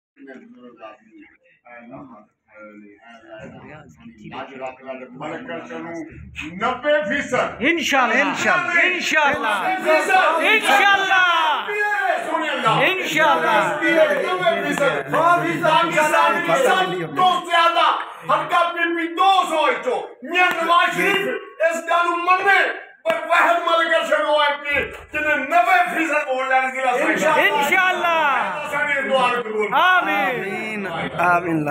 اہلیان حلقہ کمیر محمد ارشد ملک انشاء اللہ ایم پی اے پی پی 200 کی کامیابی کے لیے دعائیں کرتے ہوئے
Muhammad arshad malik mpa official pp 200 Sahiwal
Muhammad arshad malik mpa official pp 198 Sahiwal